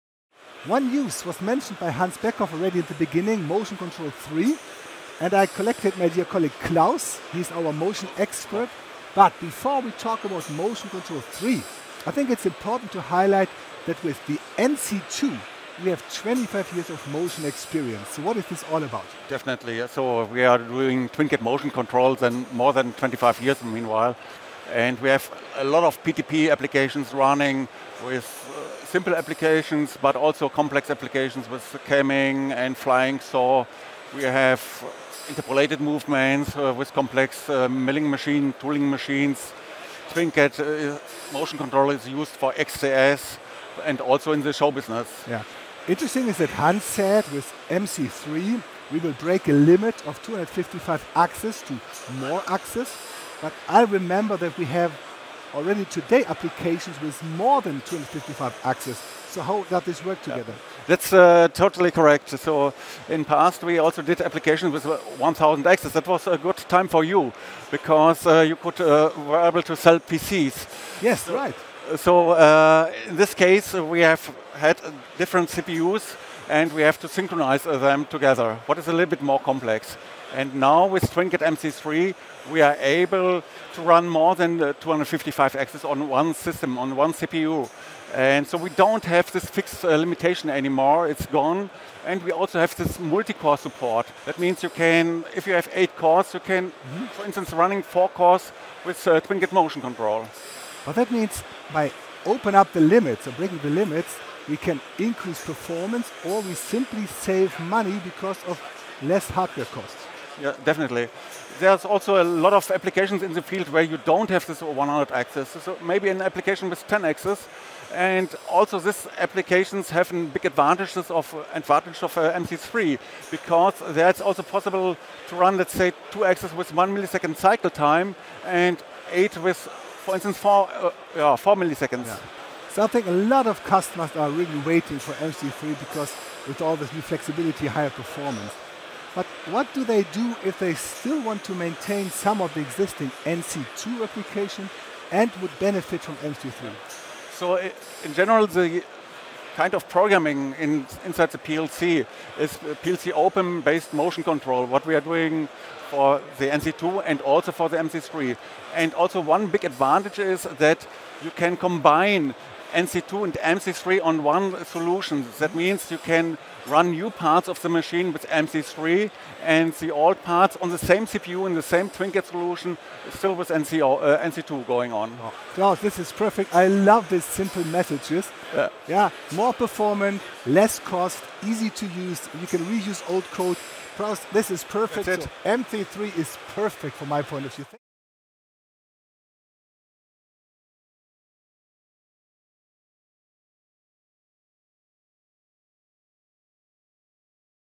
TwinCAT Motion Control 3
TwinCAT MC3 constitutes the next generation of motion control. All the brilliant features of the previous TwinCAT NC2 motion control solutions are also present in this new generation. TwinCAT MC3 is completely integrated into the TwinCAT system.
Learn more about application examples and advantages in the video or on our